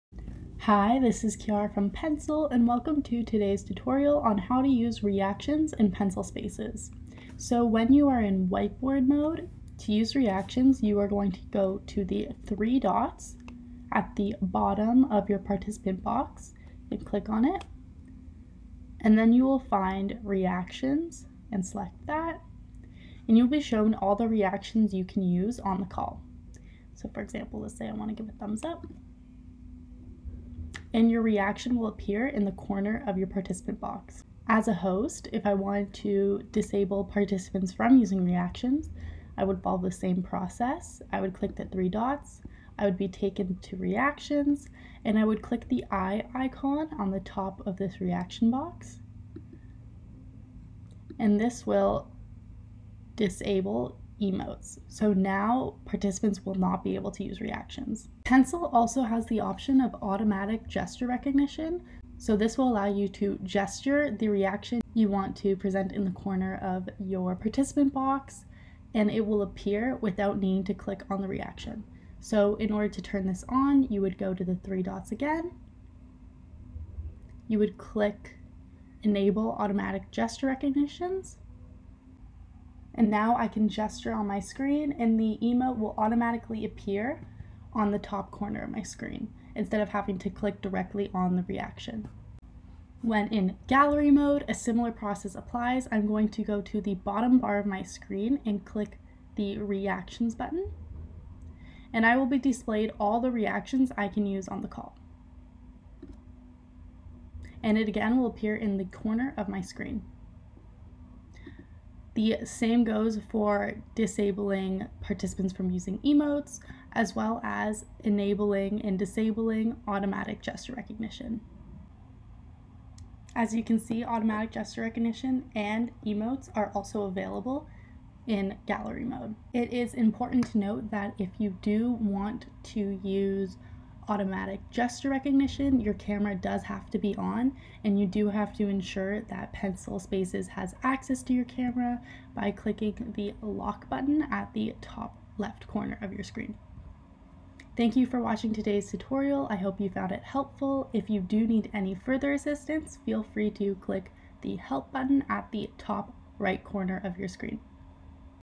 How to use Reactions in Pencil Space | Pencil Spaces Tutorials
Pencil Spaces is the perfect virtual classroom or study space. With unlimited saved whiteboards, built-in video calling and so much more, it's everything you need all in one place.

Learn how Pencil Spaces helps teachers at PencilSpaces.com.